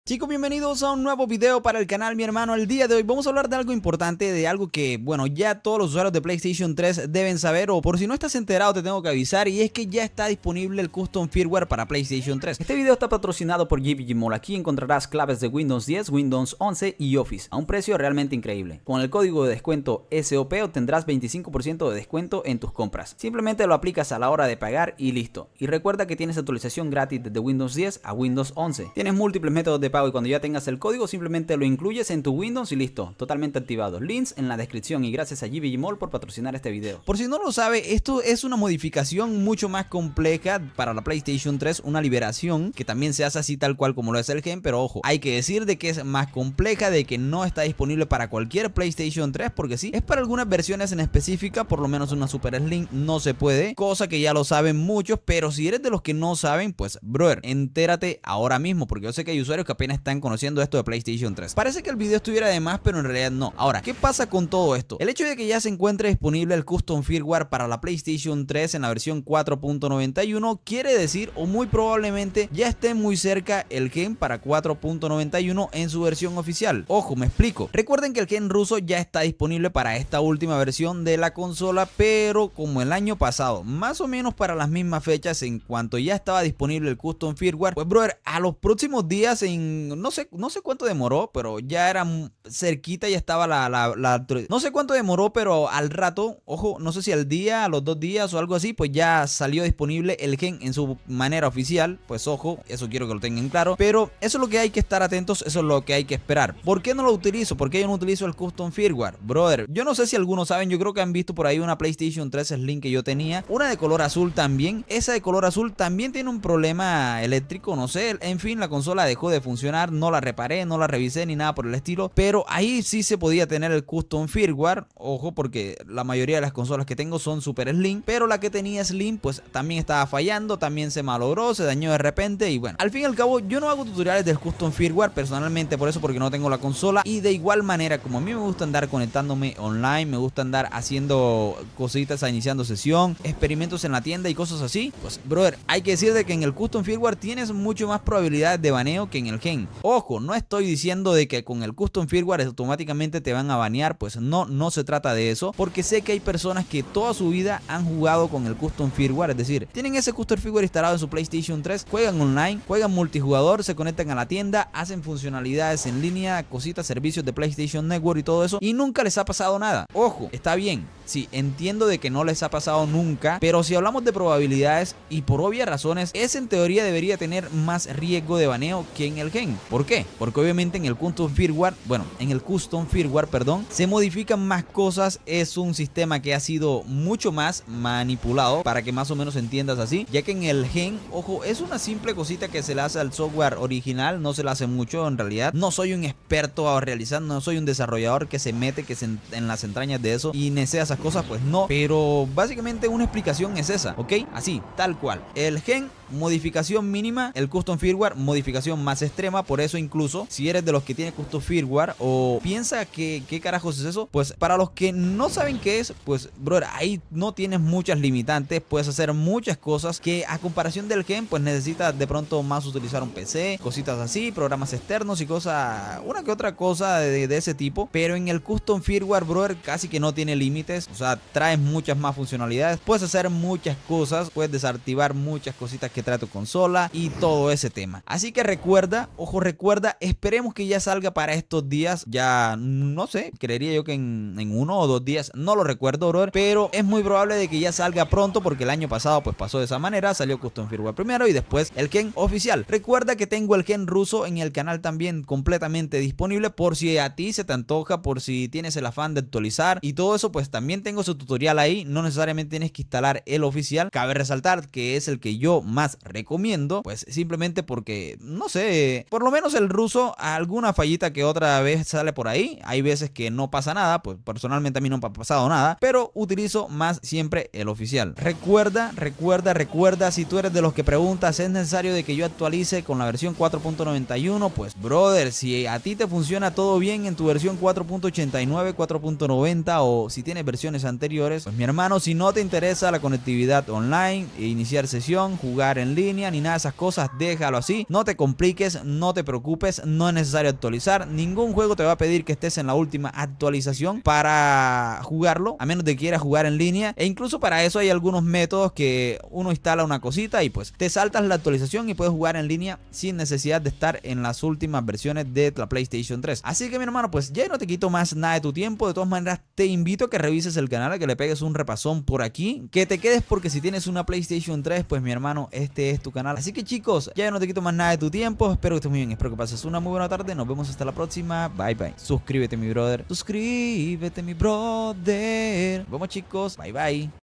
EL H4CK de PS3 4.91 Mas COMPLETO está DISPONIBLE✅
CHICOS YA ESTA DISPONIBLE EL HACK MAS COMPLETO PARA LA PS3 O PLAYSTATION 3, SI HERMANO YA LO PUEDES APLICAR A TU CONSOLA Y SACARLE EL MAXIMO PROVECHO A TU CONSOLA LIBERANDOLA Y EXPRIMIENDO ESE SOFTWARE MODIFICADO O CFW COMO SE CONOCE EN LA VERSION 4.91 DEL SISTEMA DE LA PS3

CLAVES DE WINDOS BARATAS LEGALES
GVGmall 25% code : SOP